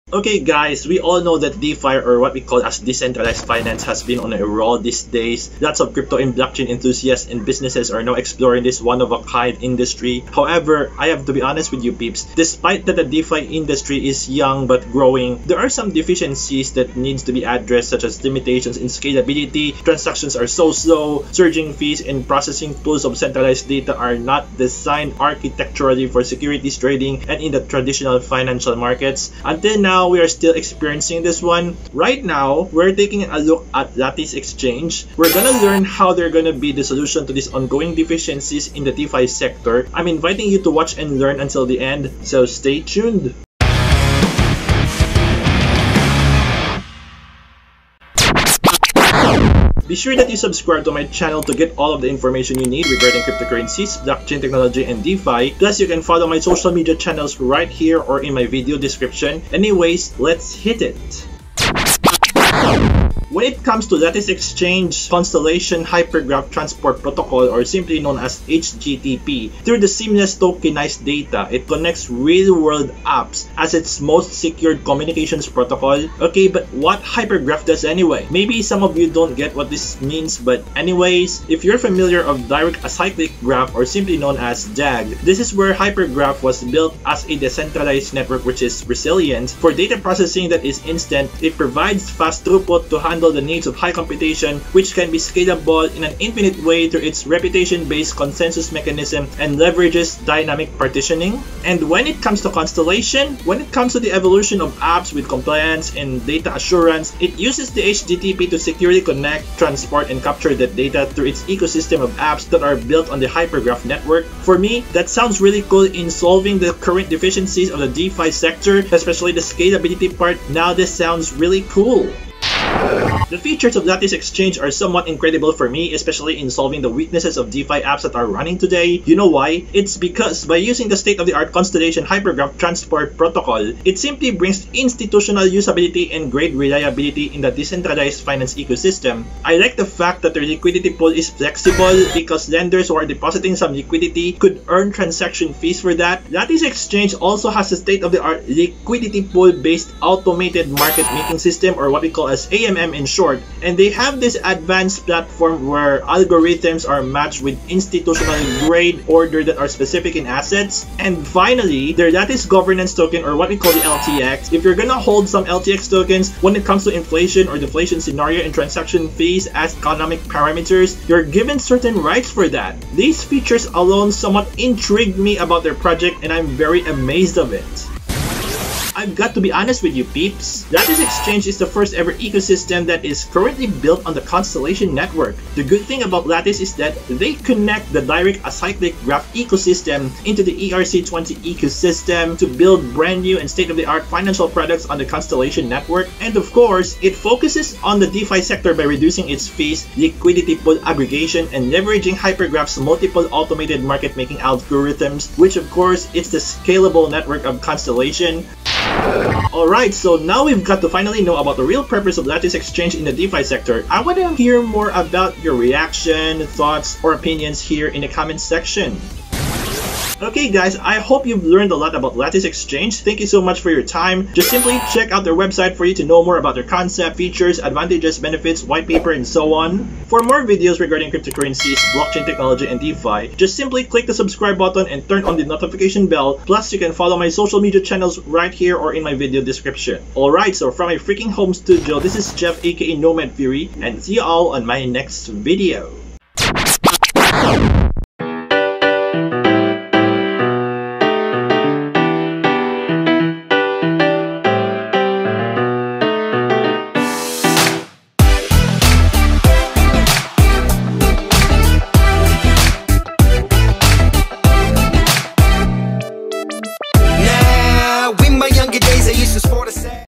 Getting to know more about the Lattice Exchange and the Hypergraph (DAG)
I'm here once again peeps! This time, we're gonna take a look at Lattice Exchange. I'm so eager to learn more about their unique project concept because of using the Hypergraph Transfer Protocol from Constellation and how it would change the game in the DeFi industry. If you wanna learn more about it, I suggest that you should watch my video until the end.

Bounty0x username: cheezcarls